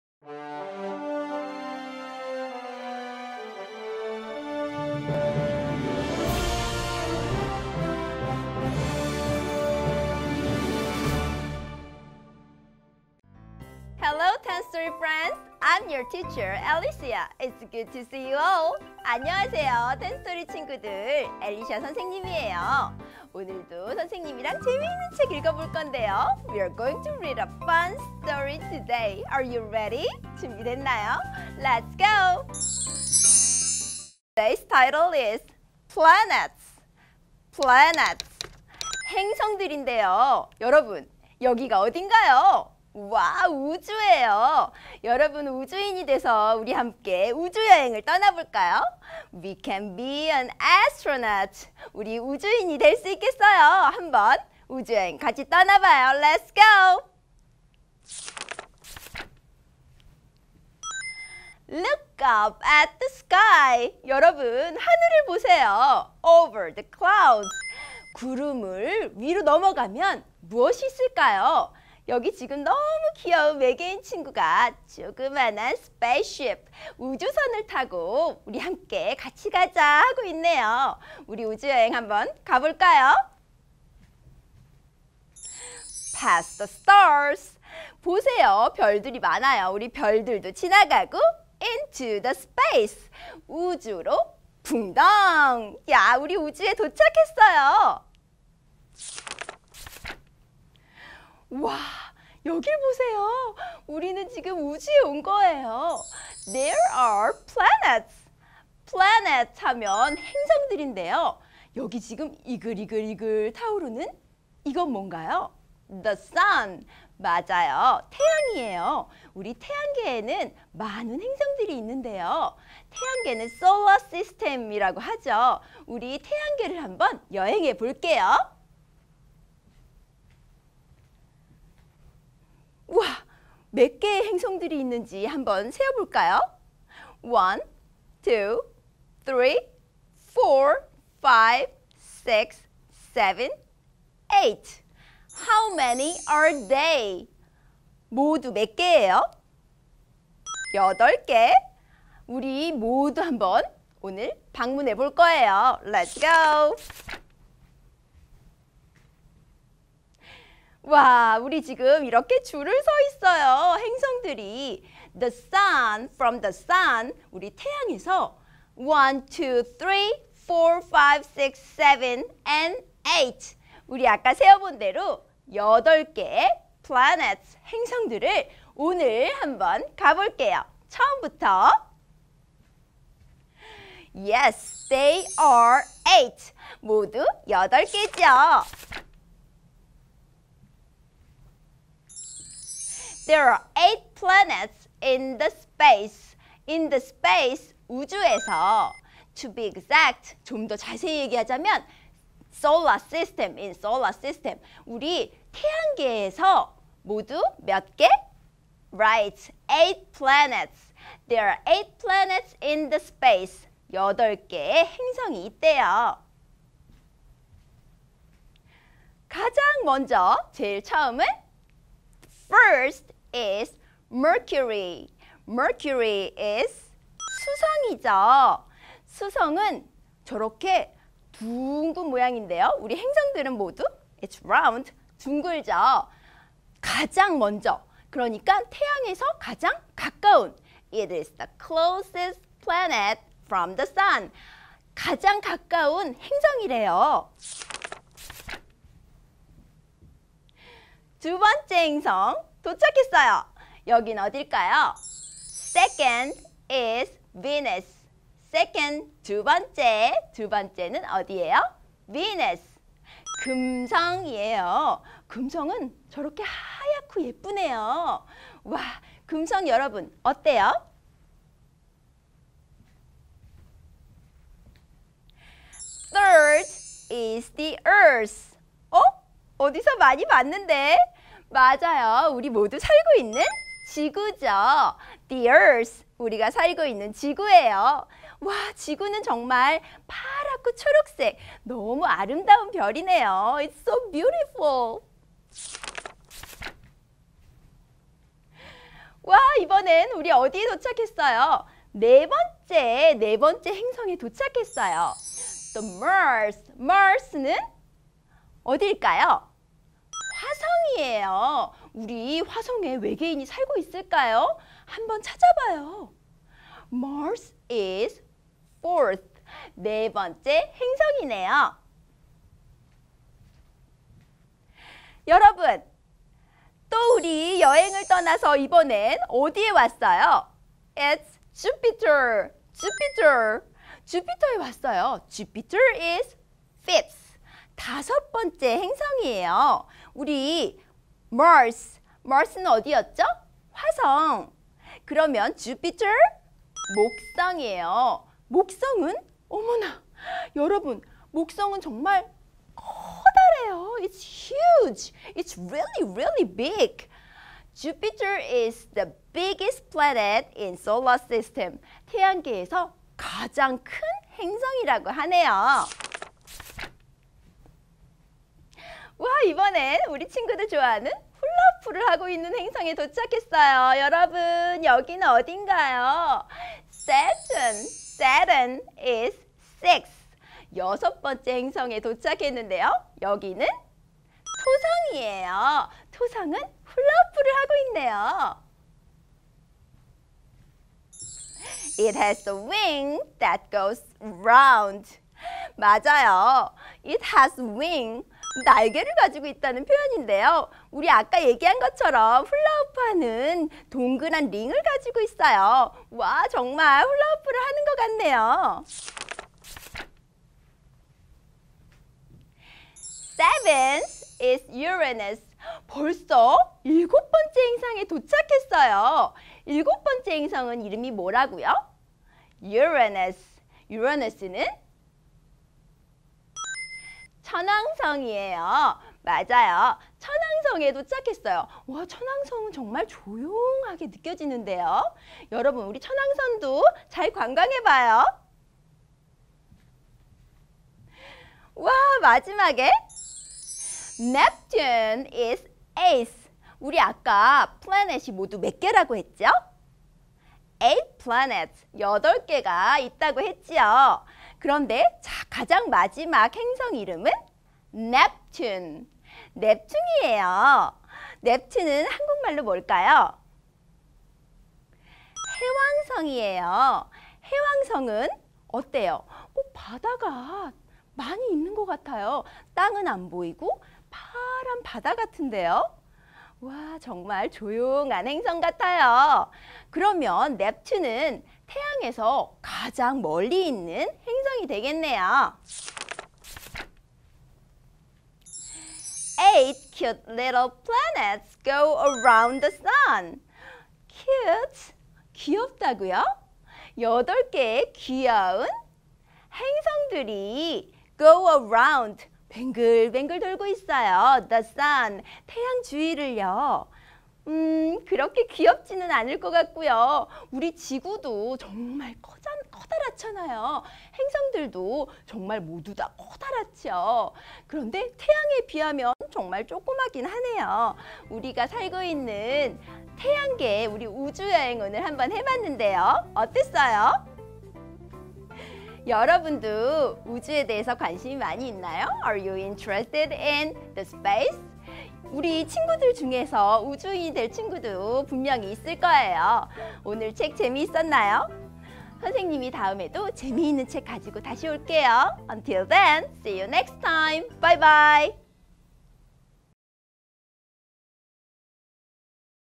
PLANETS - 영어동화책으로 재미있게 배우는 영어 자기주도학습 [ 텐스토리, 동화스터디]
우주에는 수 많은 별들이 있어요. 그중 행성들의 이름과 특징을 알아보아요